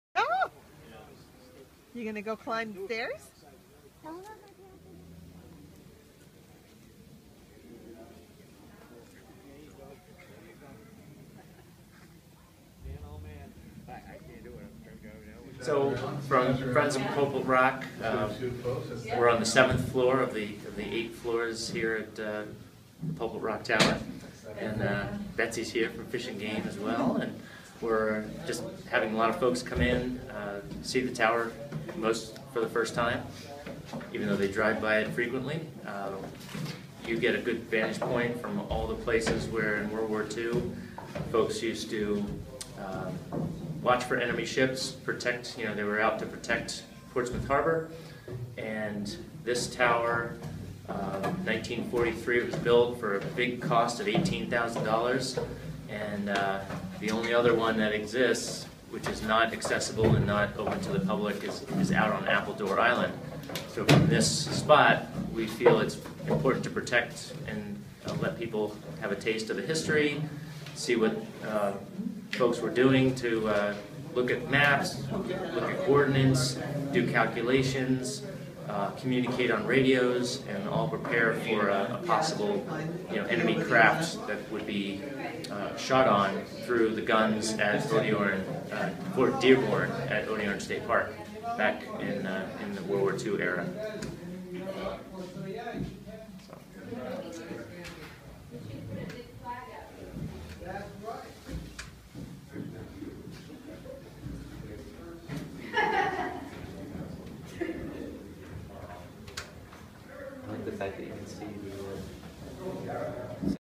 Pulpit Rock Tower Rye, NH
Ray Panek of the Friends of Pulpit Rock Tower talks about the history of the observation tower built during WWII to protect the N.H. seacoast.